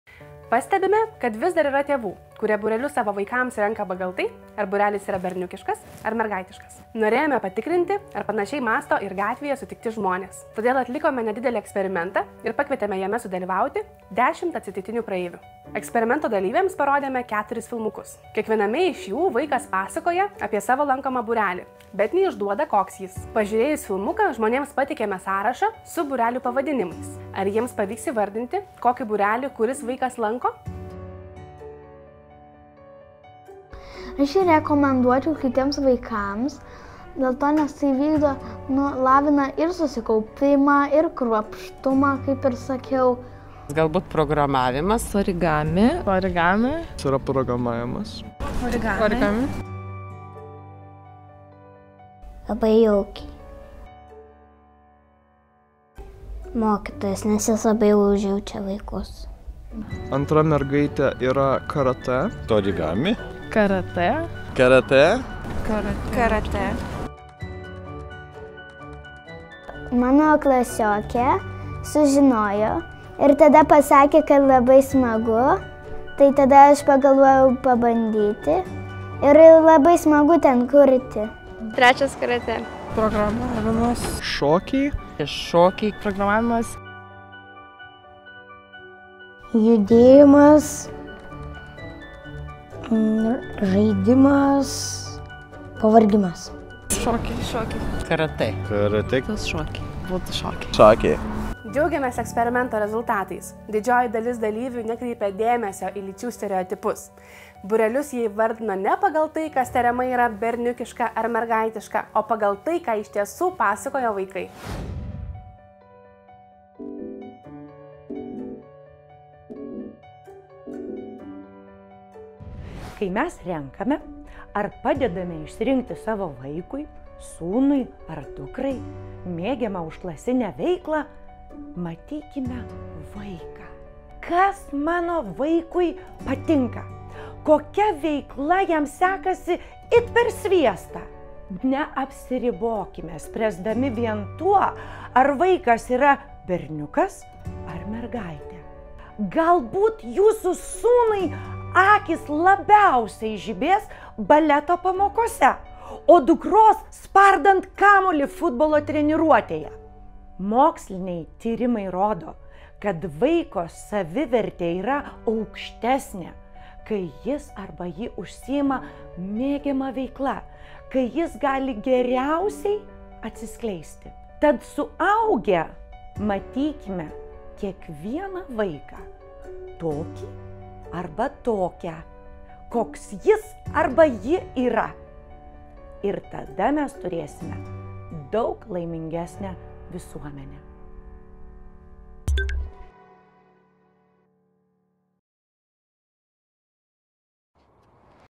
Tvirta mergaitė. Būreliai
Rinkdami užklasinę veiklą matykime vaiką - kas mano vaikui patinka, kokia veikla jai / jam itin gerai sekasi.
Moksliniai tyrimai rodo, kad vaiko savivertė yra aukštesnė, kais jis / ji užsiima mėgiama veikla.